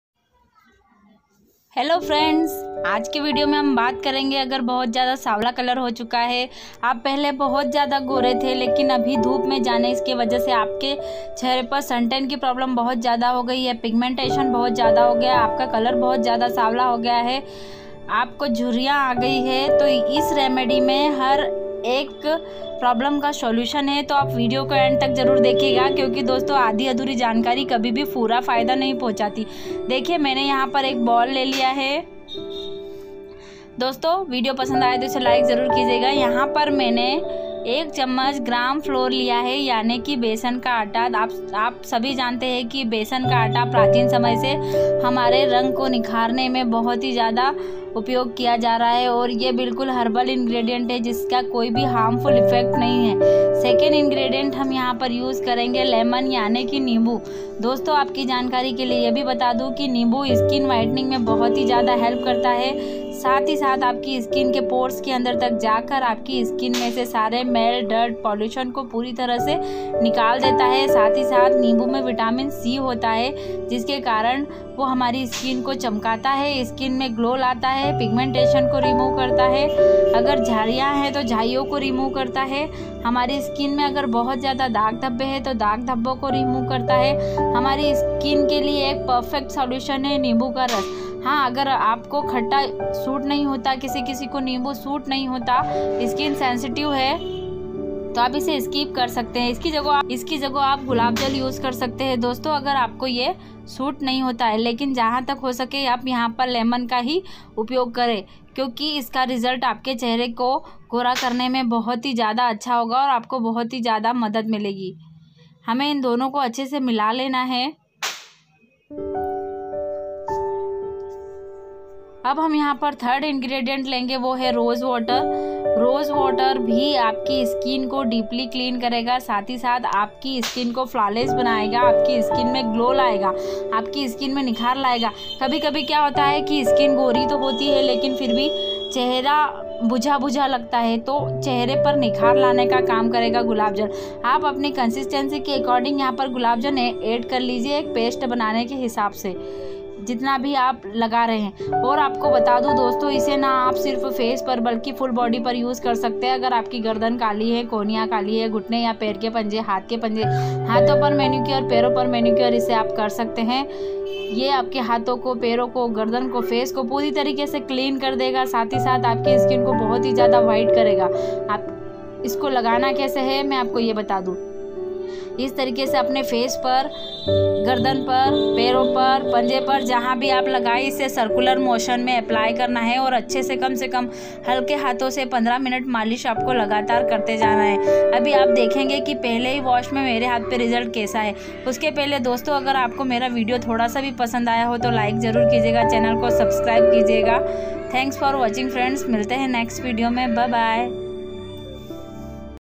skin whitening at home // gore hone ka gharelu nushkha // how to remove pigmentation, wrinkless
Hello freinds
@Priti creation  Today I will show you